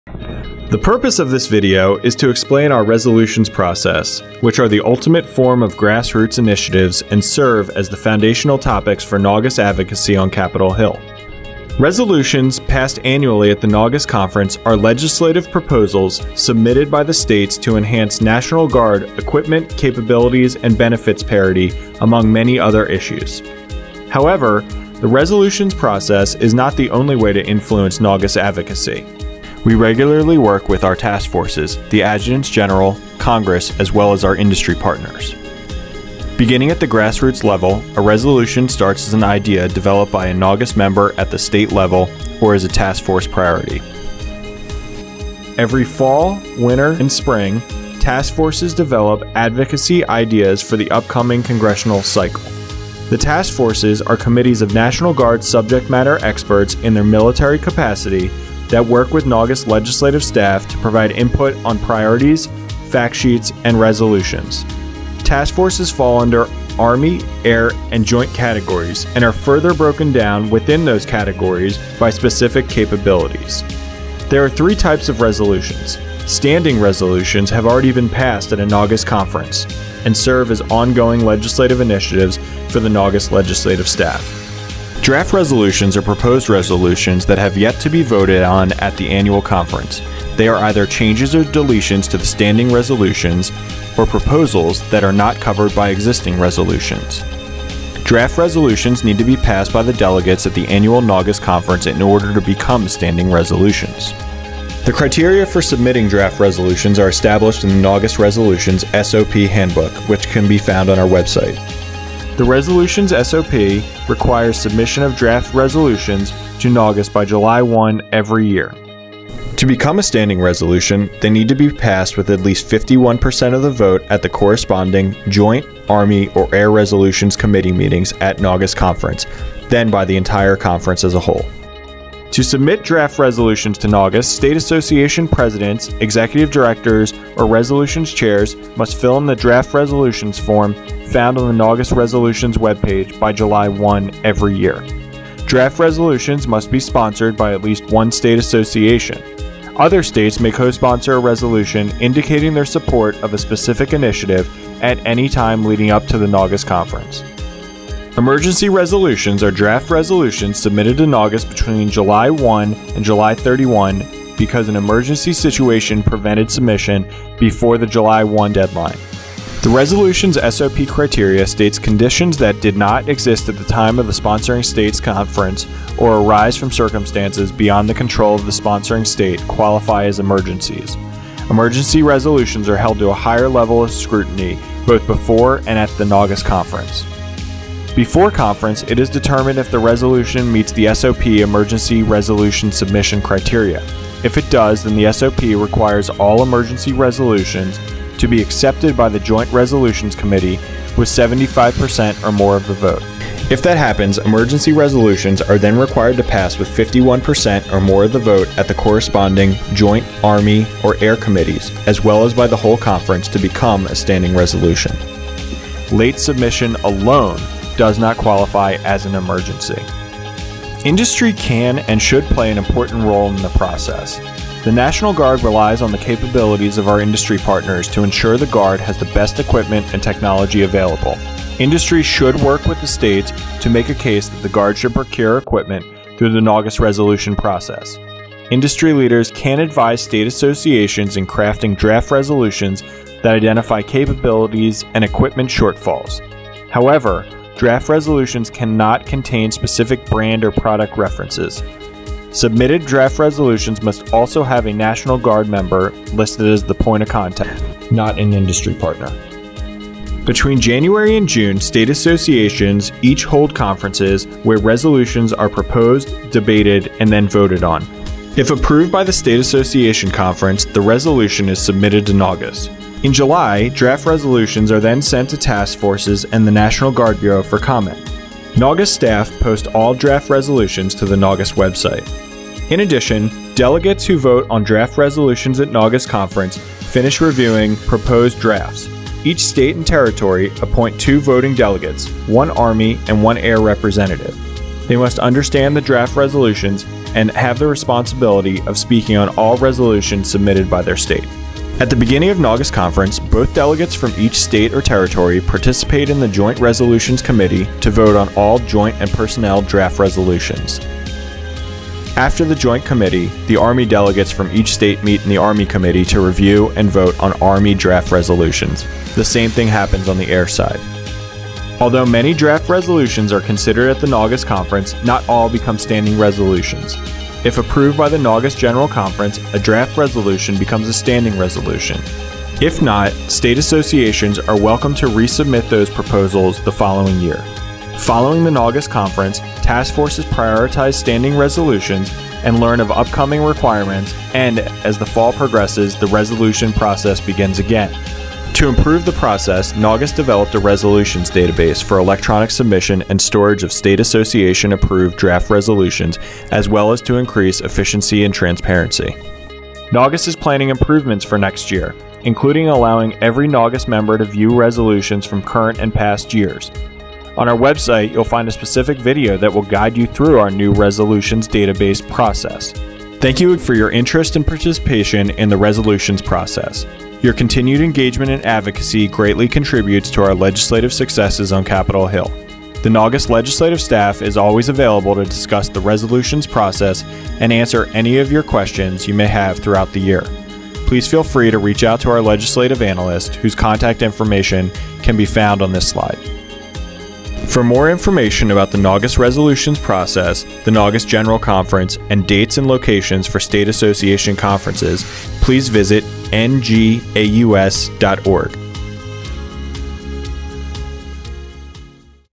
The NGAUS Resolutions Process
This video explains how grassroots initiatives become foundational topics for NGAUS advocacy on Capitol Hill.